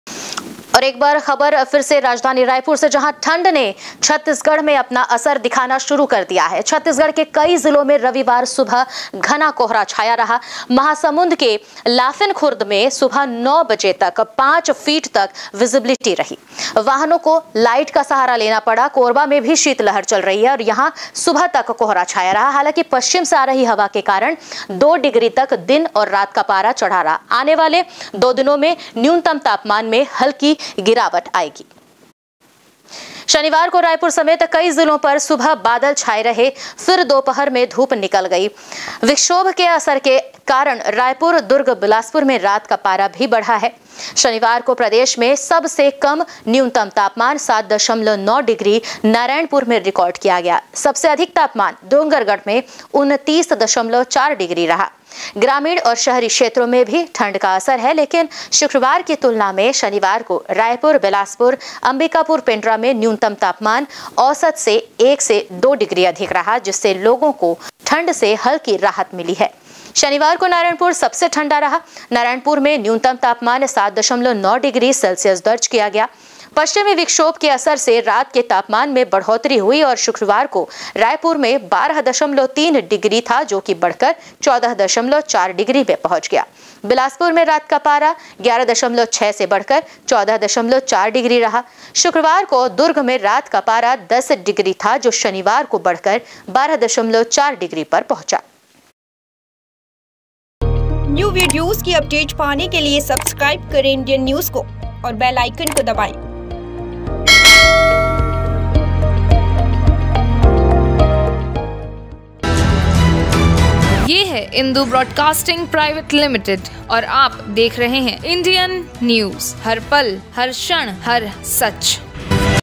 Raipur समेत कई जिलों में छाया घना कोहरा, पड़ रही कड़ाके की ठंड
Our Links